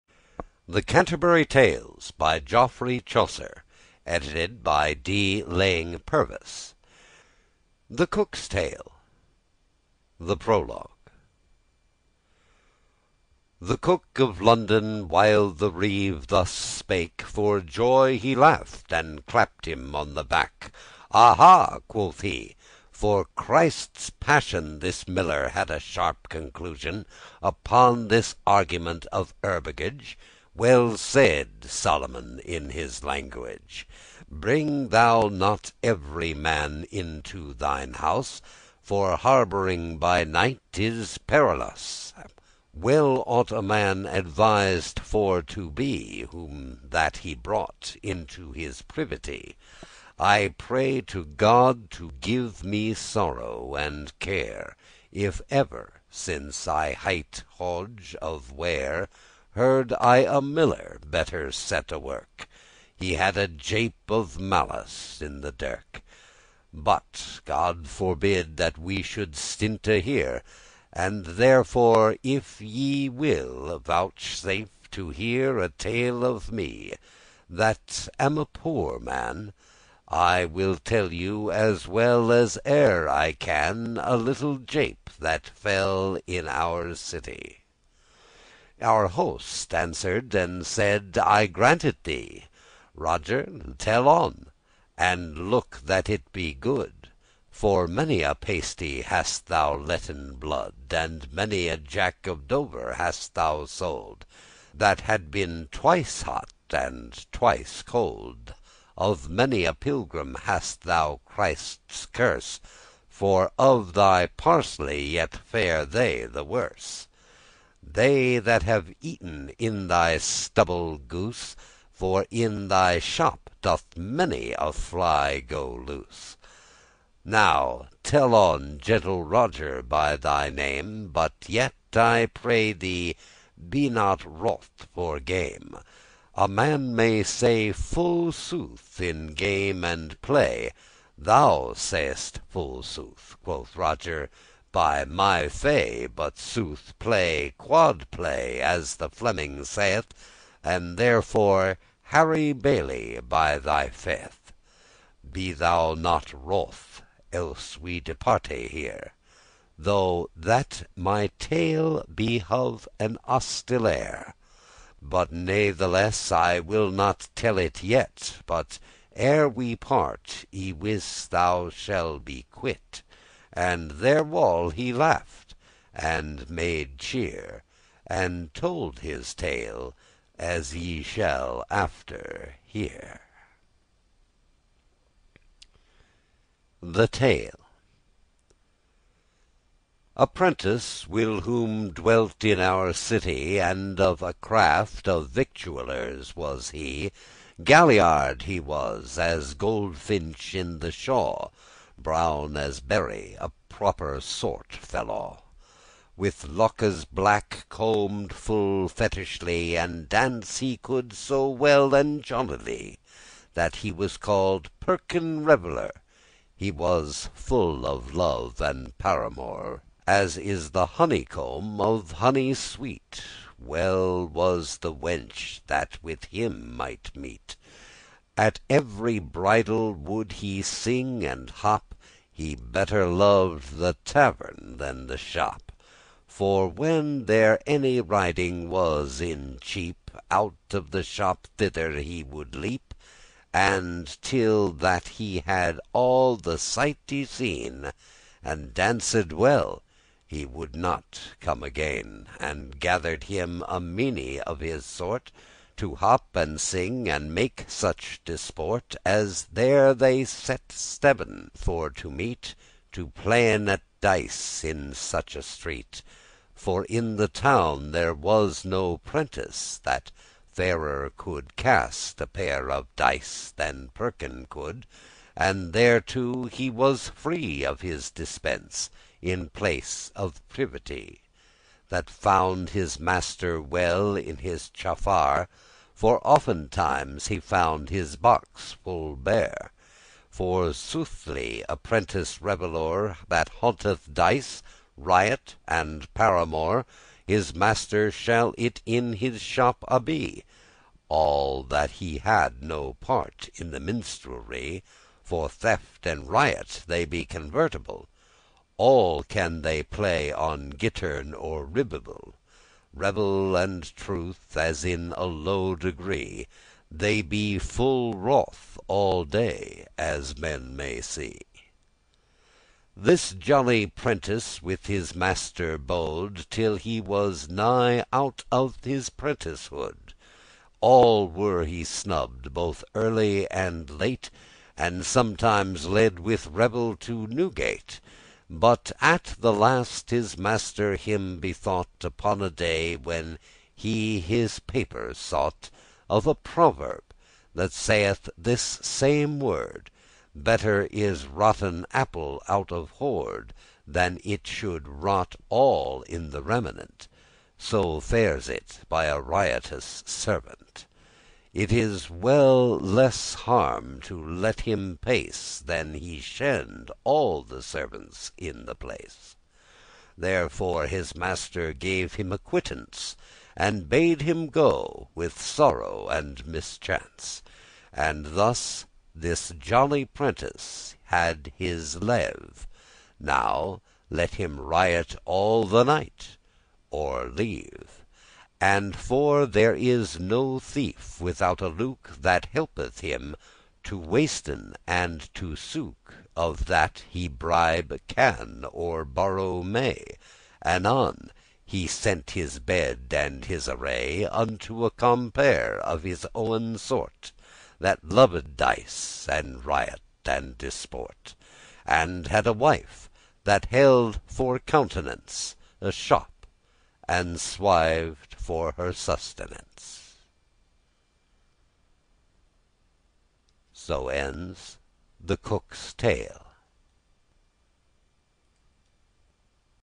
Chapter7 of The Canterbury Tales by Geoffrey Chaucer- The Cook's Tale - English Learning
The Canterbury Tales is a collection of stories written in Middle English by Geoffrey Chaucer in the 14th century (two of them in prose, the rest in verse). The tales, some of which are originals and others not, are contained inside a frame tale and told by a group of pilgrims on their way from Southwark to Canterbury to visit the shrine of Saint Thomas Becket at Canterbury Cathedral.

story reading| free audible| Complete Lit| YouTube| Audiobook with Subtitle| Online book| Best Online Book